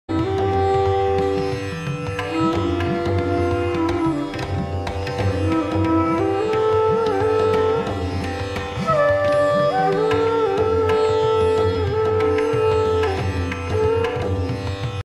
seruwawila mangala maha seya
It was built during the reign of King Kavan Tissa, Prince of Ruhuna, (2nd century BC) containing the Lalata Dathun Wahanse (sacred forehead bone) of Buddha. It can be reached by land and sea. The sea route begins at Trincomalee to Muttur on boat and another 16 km by roads and the land route is via Kantale, to Allai which is approximately 45 km through dense forest.

According to Richard Leslie Brohier, the Seruwila region was a vast swamp or villu where the flood waters of the Mahaweli Ganga collected. This villu was the home of large flocks of teals (seru) during migratory period. That perhaps was how the place came to be known as Seruwawila.